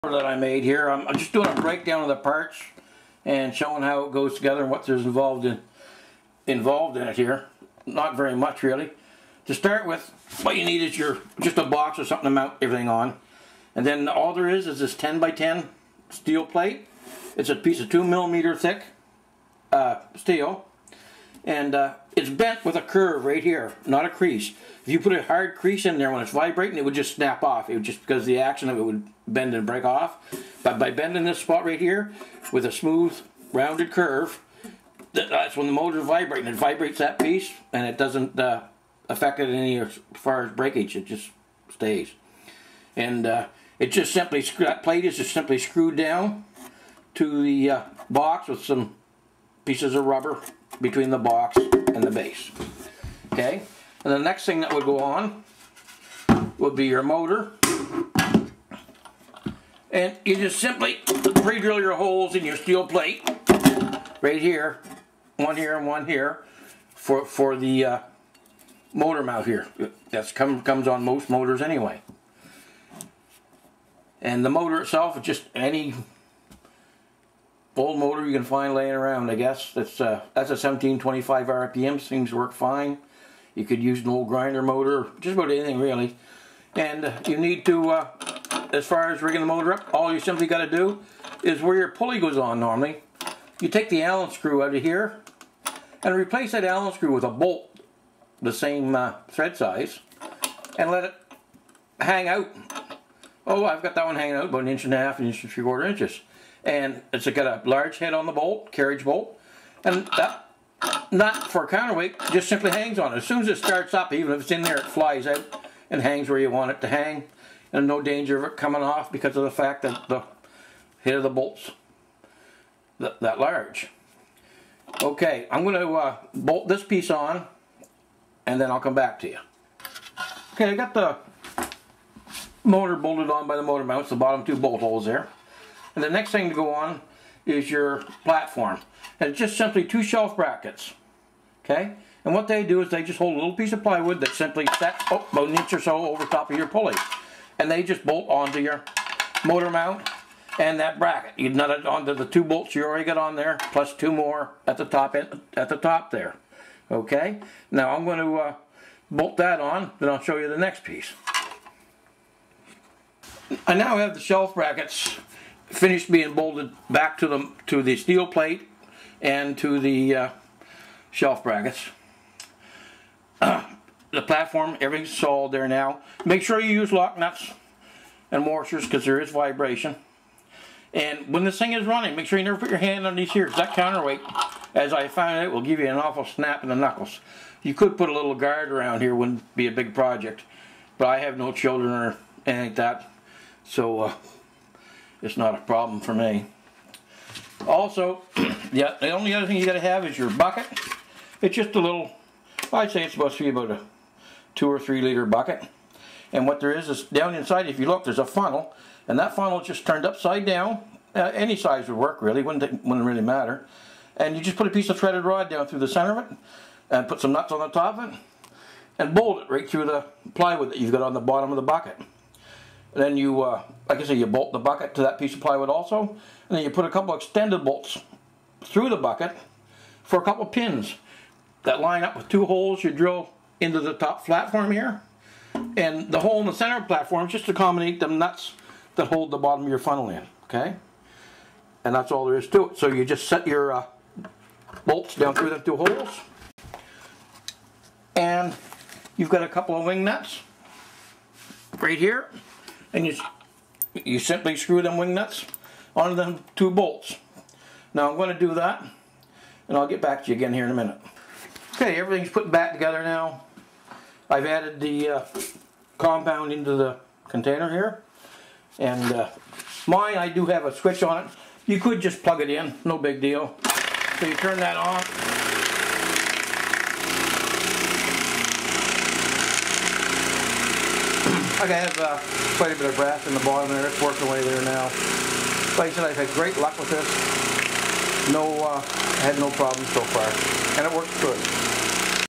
How to make a Brass tumbler details
A few people asked for more detail on  tumbler so  this  video is showing how it is made and the pieces being put together, I hope this helps , This tumbler is made from parts found just laying around my workshop and most people would have this stuff ,if not what you would have to get could easily got at most scrap yards,if you make one of these let me know how it works for you.